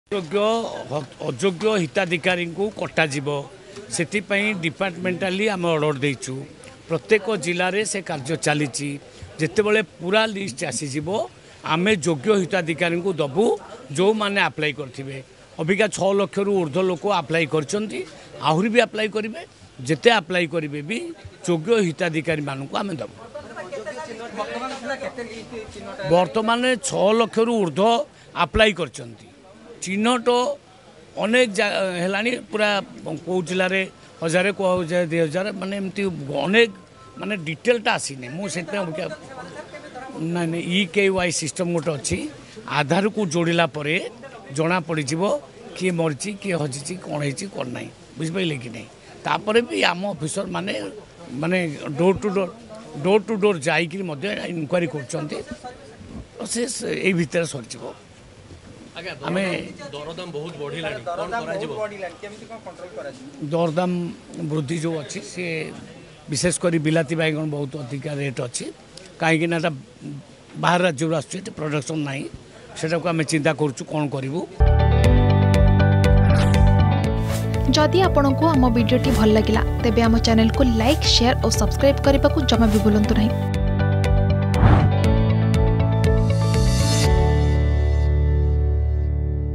ଅଯୋଗ୍ୟ ହିତାଧିକାରୀଙ୍କୁ କଟାଯିବ || Krushna Chandra Patra , BJP
ଅଯୋଗ୍ୟ ହିତାଧିକାରୀଙ୍କୁ କଟାଯିବ ଏବଂ ଯୋଗ୍ୟ ହିତାଧିକାରୀଙ୍କୁ ଦିଆଯିବ : କୃଷ୍ଣ ଚନ୍ଦ୍ର ପାତ୍ର, ଯୋଗାଣ ମନ୍ତ୍ରୀ